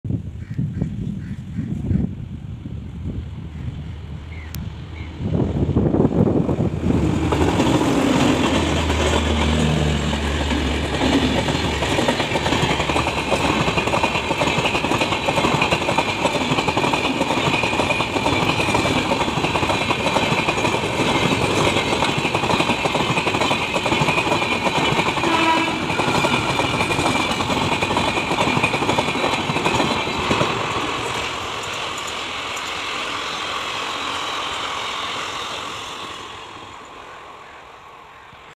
OFFICIAL PARKING LIGHT😈❤️ | CHENNAI CENTRAL SUPERFAST EXPRESS | WAP-7
OFFICIAL PARKING LIGHT FOR RAILFANER😈❤️ | CHENNAI CENTRAL SUPERFAST EXPRESS | WAP-7

LOCATION : DIVA JUNTION CABIN
LOCOMOTIVE : WAP-7
TRAIN : CHENNAI CENTRAL SUPERFAST EXPRESS
DATE SHOT : 5 FEBRUARY
EDITING APP : INSHOT
TRAIN JOUNREY START : CHENNAI CENTRAL
TRAIN JOURNEY END : MUMBAI LLT